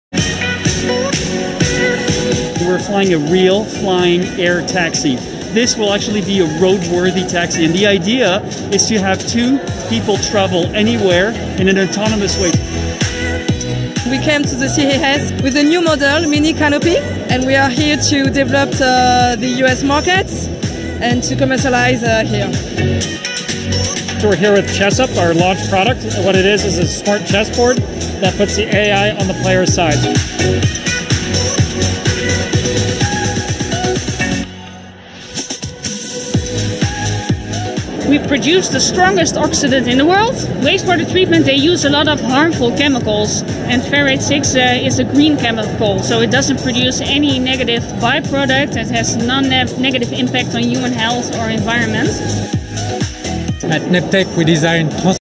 Startups Shaping The Future CES 2022
Startups Shaping The Future Since the beginning of time, humans have dreamed of a better tomorrow. From the first stone tools to the first computers, we’ve imagined new ways to improve our lives. Today, we’re in the midst of a new revolution – the startup revolution. Entrepreneurs are using technology to disrupt the way we live, work, and play.
CES 2022